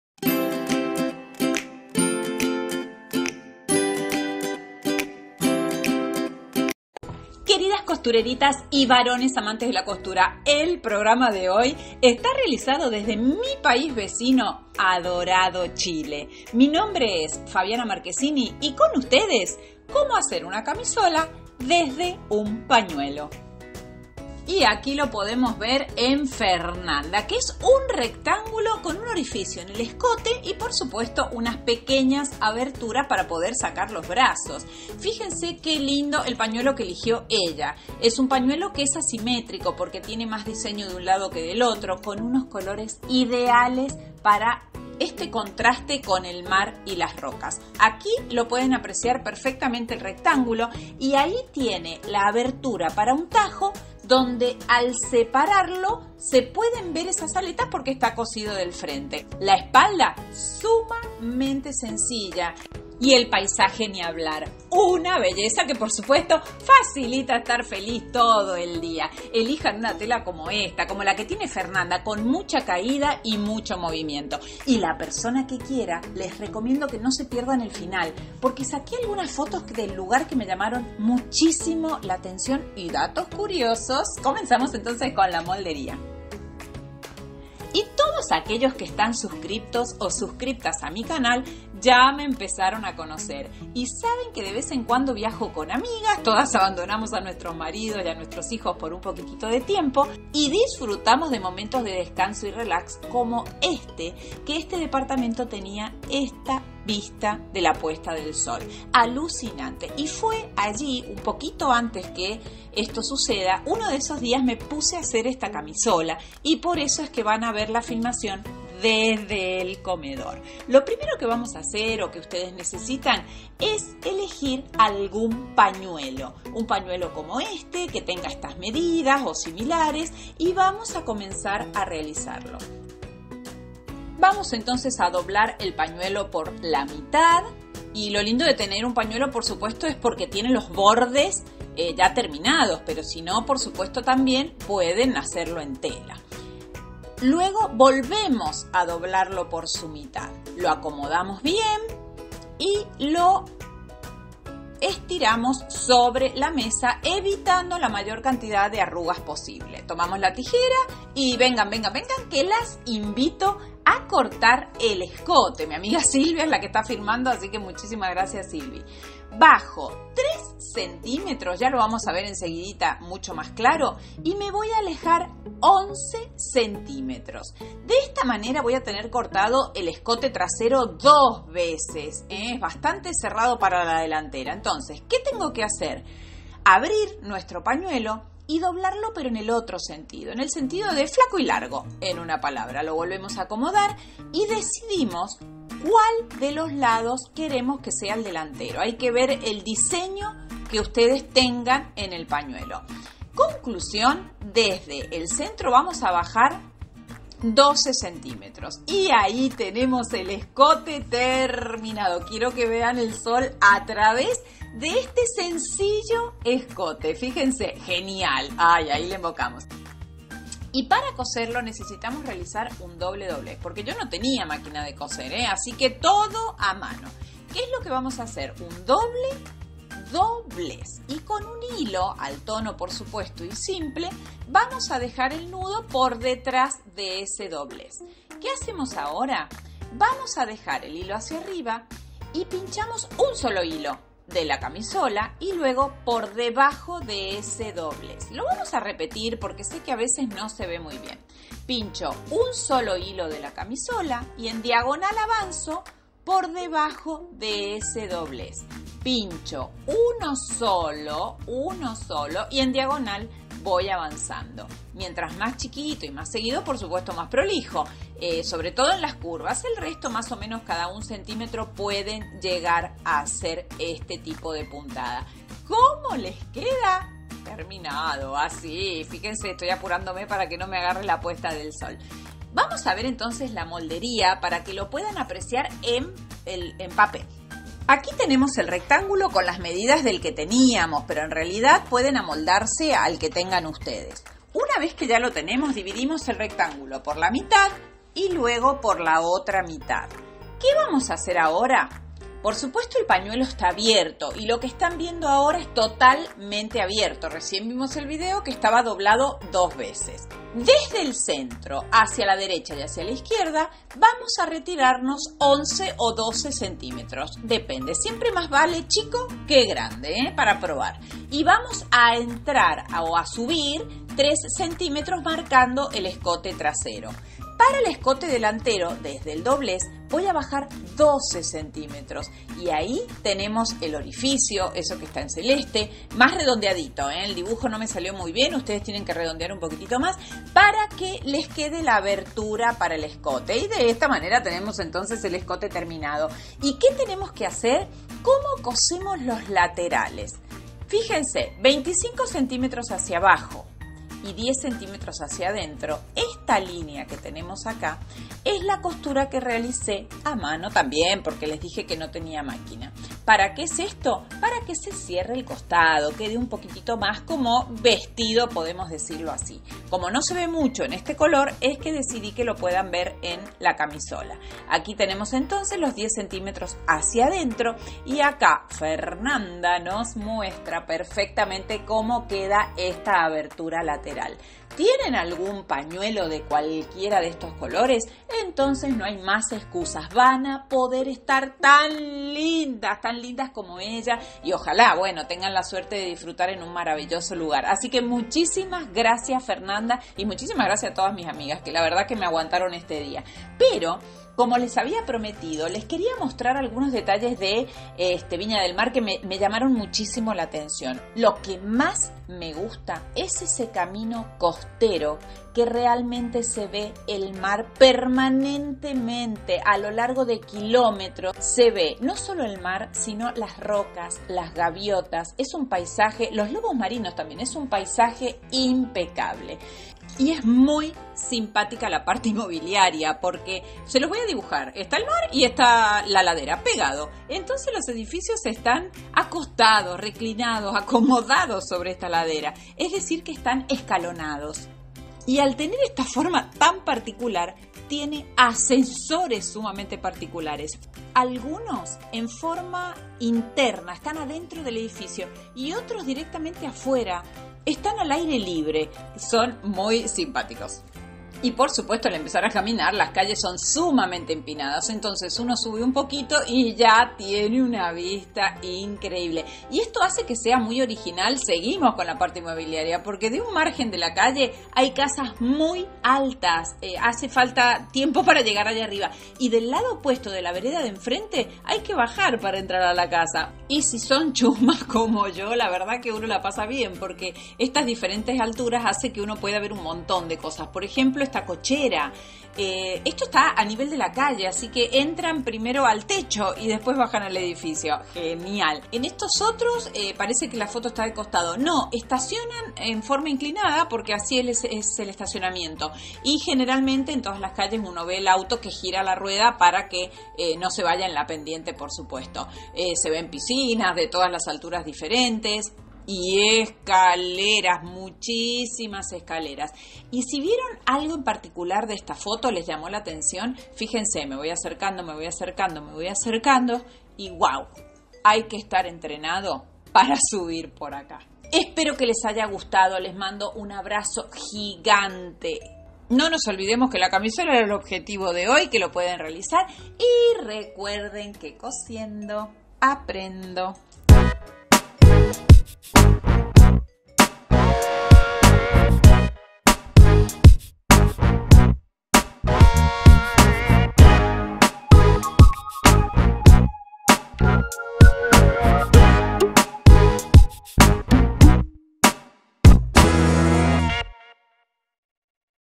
Cómo reciclar un PAÑUELO en una CAMISOLA - Fabiana Marquesini - 204
Tenès un PAÑUELO que no usàs? En poco pasos y sin màquina de coser, convertilo en una cómoda CAMISOLA.

Picture designed by Freepik